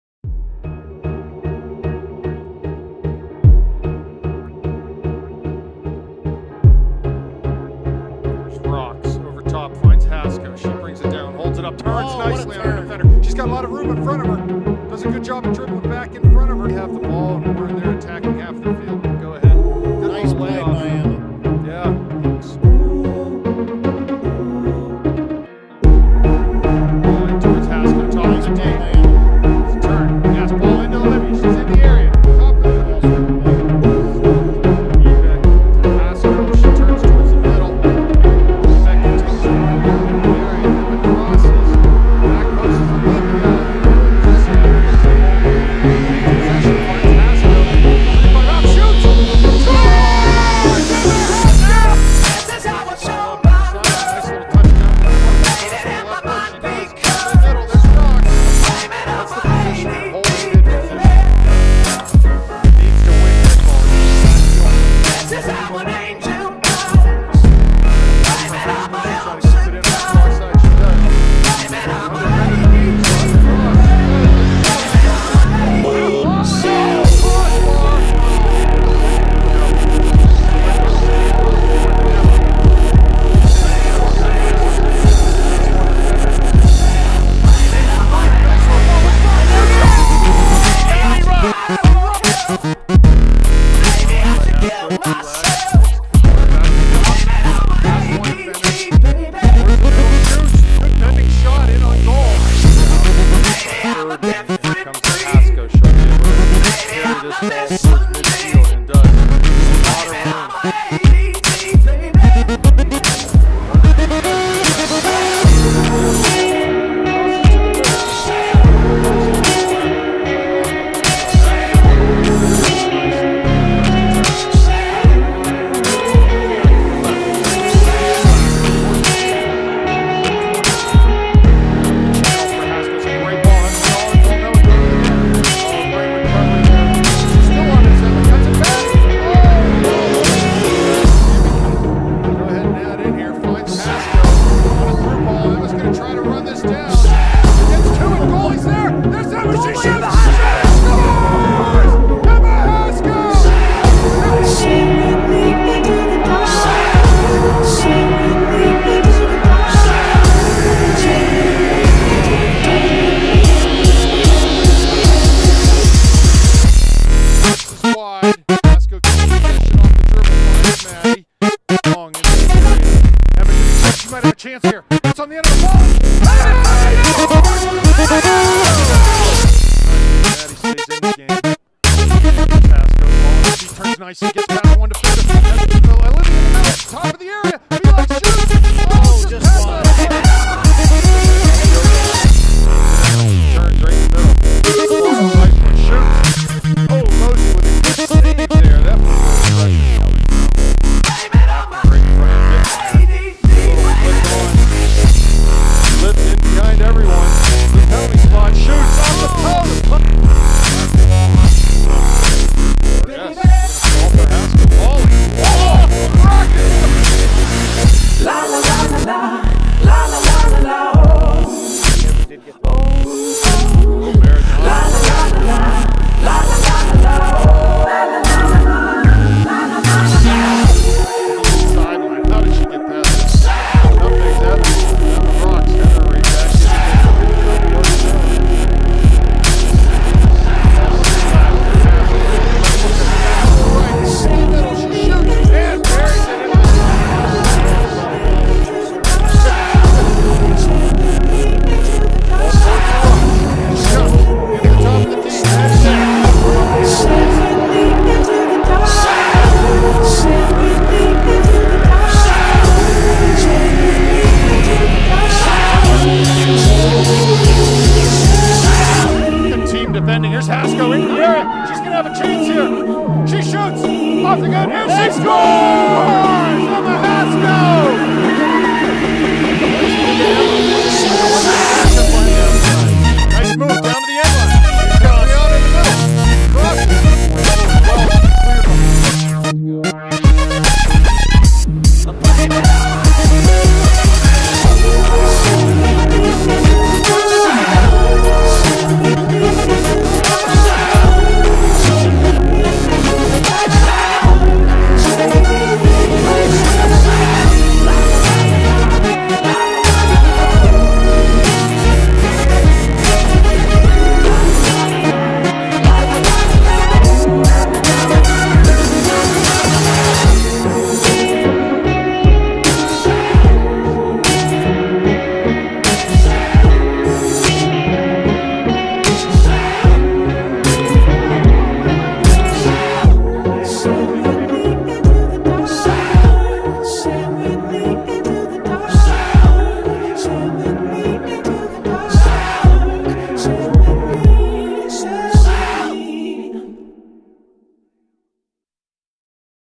Emma Hasco Highlight Video #24 Upper St. Clair
Emma Hasco
Forward - Upper St. Clair
2013 All-WPIAL Selection
13 Goals, 19 Assists